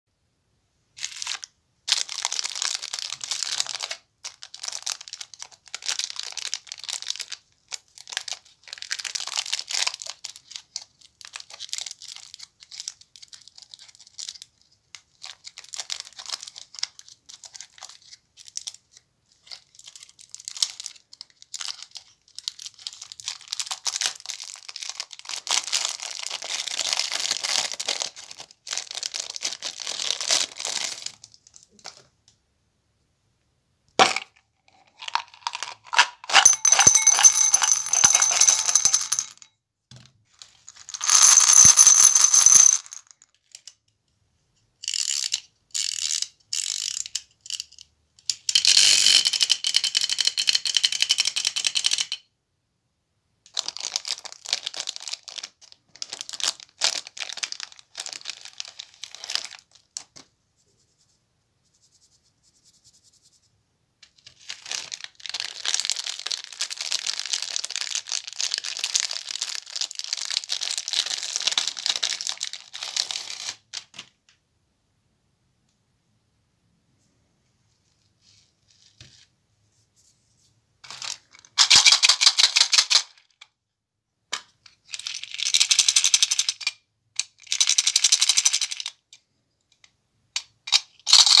Satisfying video Asmr Lollipops candy and chocolate Cutting video
A relaxing video with delicious lollipops, sweets and sweets on a stick.  I really love sweets and lollipops, just like you!